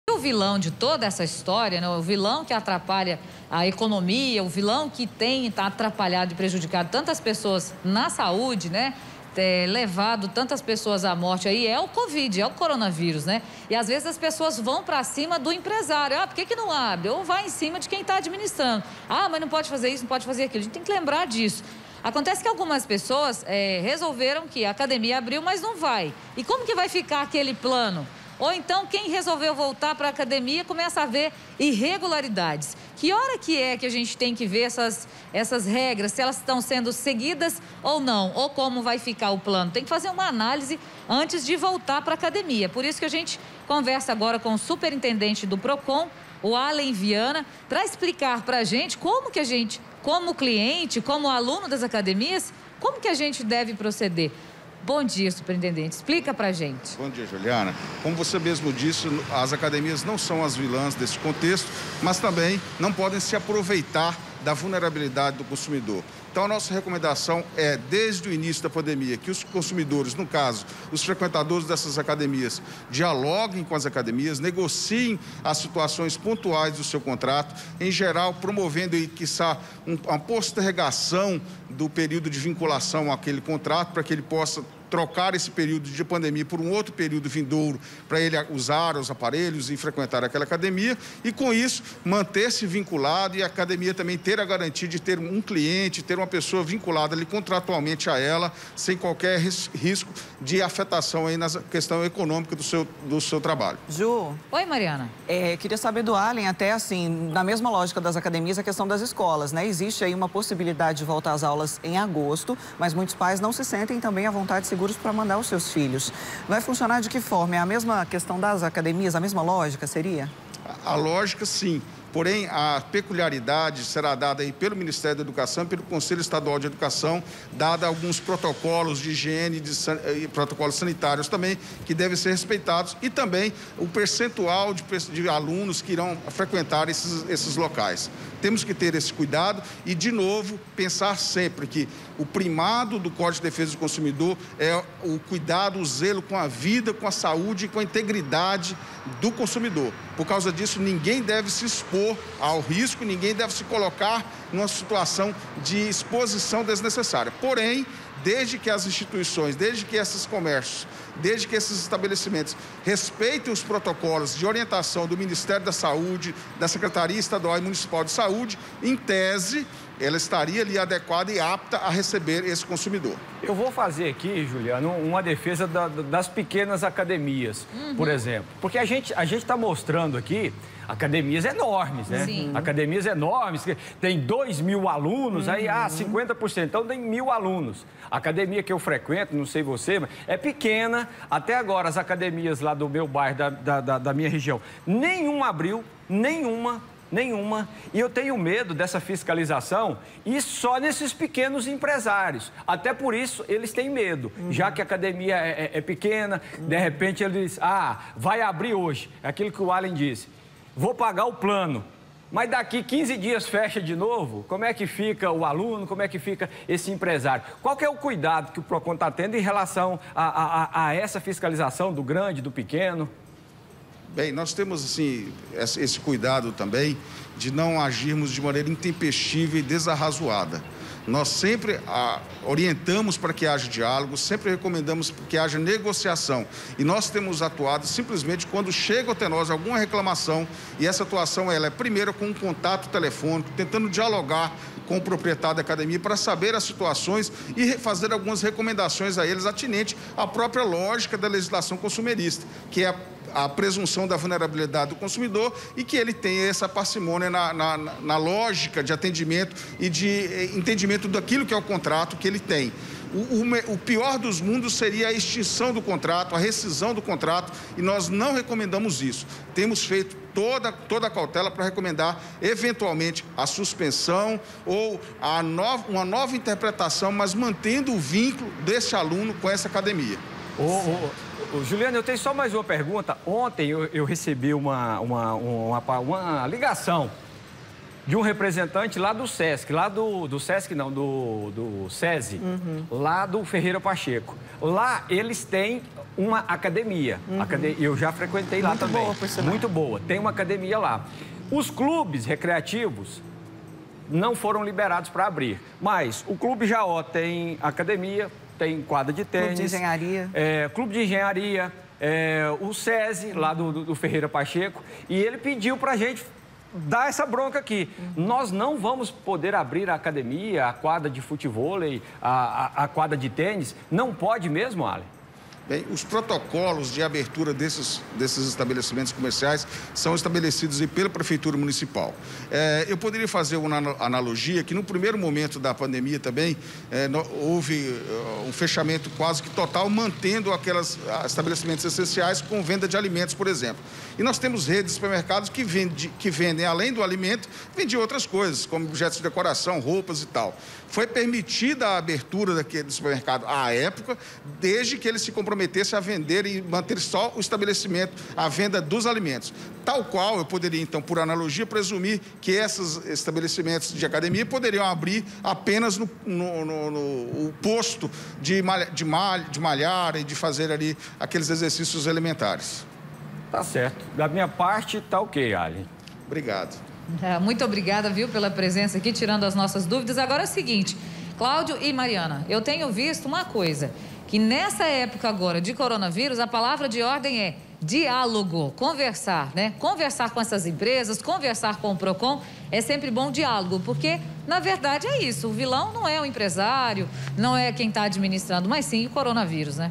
PROCON FISCALIZA ACADEMIAS QUE REABRIAM
AS ACADEMIAS, ASSIM COMO TODO COMÉRCIO REABRIAM  HÁ DOIS DIAS, MAS PROCON JÁ RECEBEU DENUNCIAS DE DESCUMPRIMENTO DAS NORMAS DE SEGURANÇA.  @RecordTVGoias